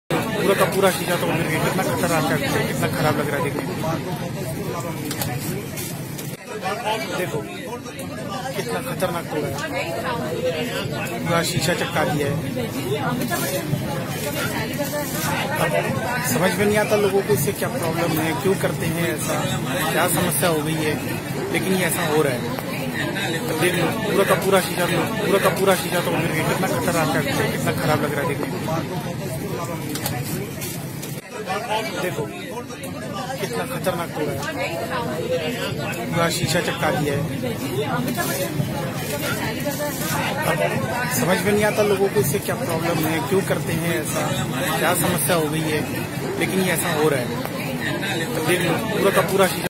Third time stones thrown on "Vande Bharat Express"
"वंदे भारत एक्स्प्रेस" वर तिसऱ्यांदा दगडफेक